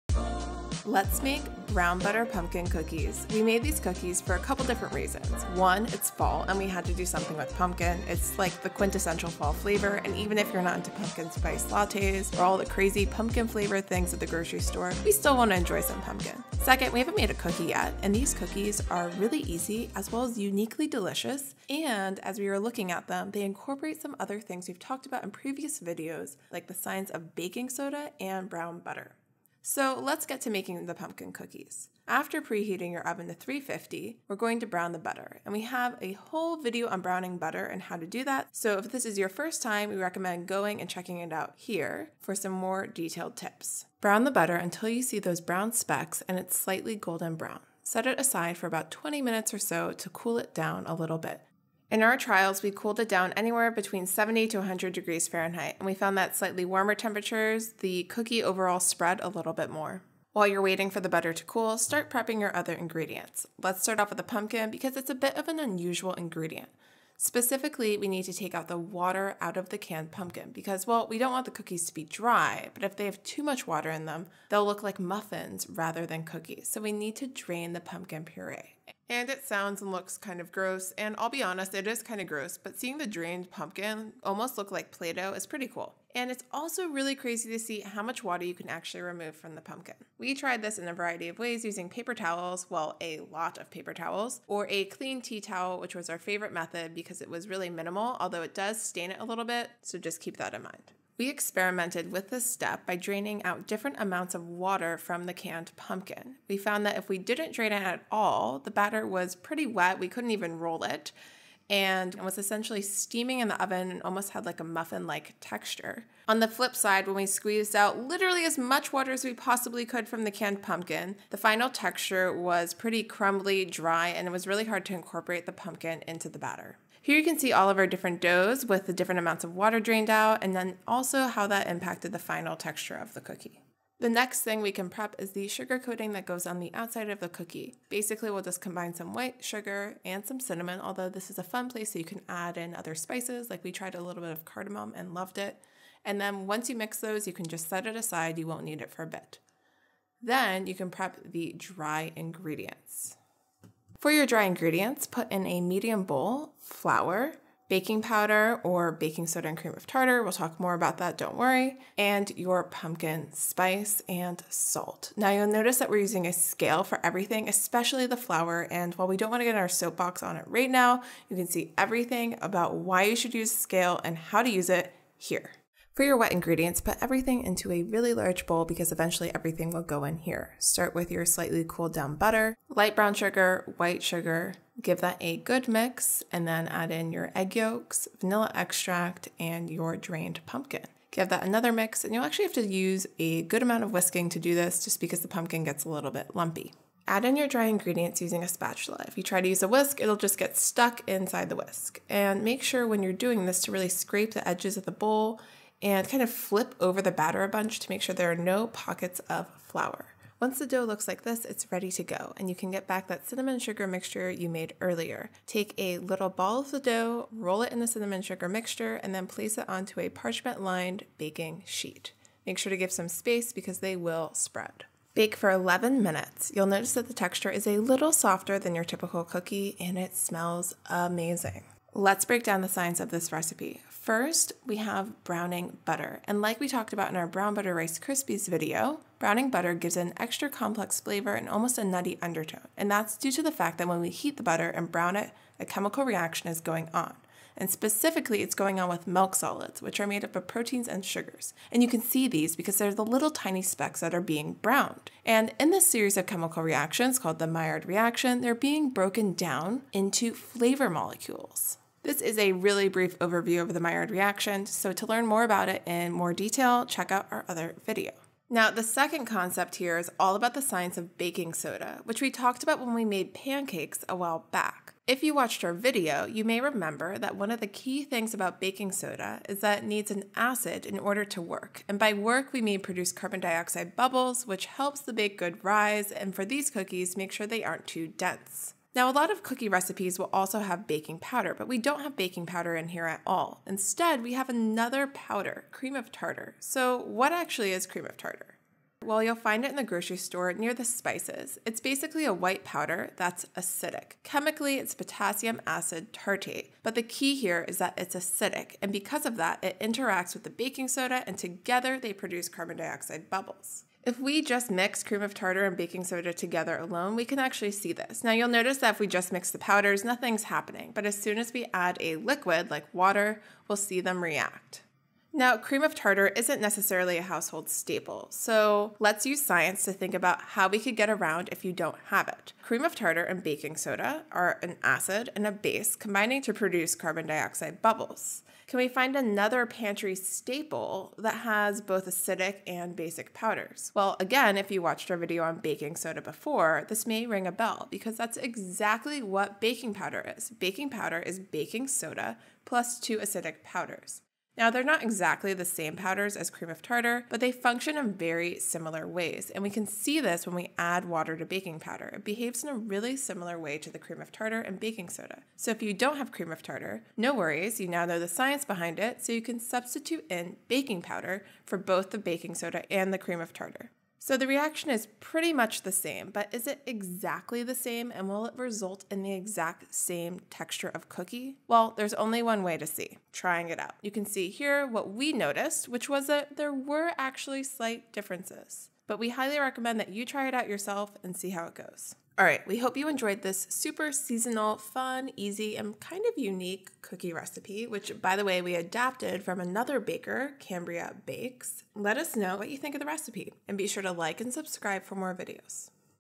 What's the difference between baking soda & baking powder? Learn Thru: PUMPKIN SNICKERDOODLE COOKIES
Make pumpkin snickerdoodle cookies to explore the science behind baking soda and cream of tartar (versus baking powder), as well as some mini-lessons on the importance of moisture and the Maillard reaction in creating the perfect flavor and texture in these seasonal cookies. The recipe we used and closely adapted is from Cambrea Bakes and can be found and you can find both it and our science-based activities using it in our supplementary materials on our website.

In this activity, you will be able to…
Explain the difference between baking soda and baking powder
Observe how the amount of water in cookie dough impacts its texture
Explain how browning butter creates flavor molecules

Find the printable instructions, as well as a lesson plan and other supplementary educational resources for this video

0:00-3:53 basic template
3:53-4:28 the science: Maillard reaction
4:28- 6:44 the science: cream of tartar and baking soda